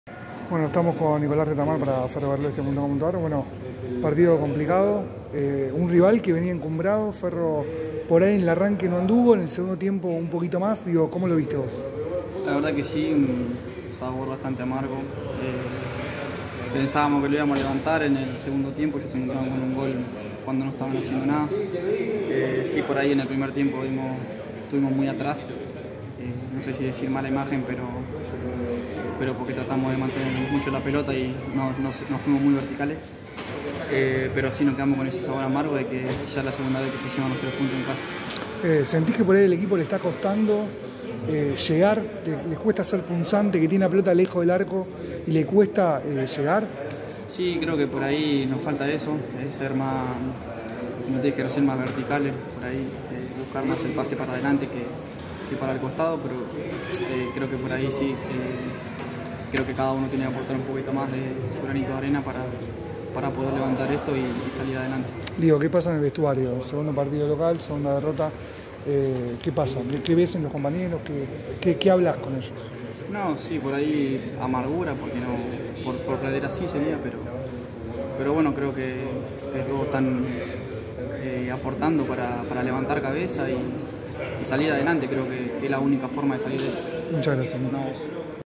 Entrevista a Nicolas Retamar
⚽️ Nicolás Retamar analiza la dura derrota de Ferro con Estudiantes por 2 a 0

05/03/2023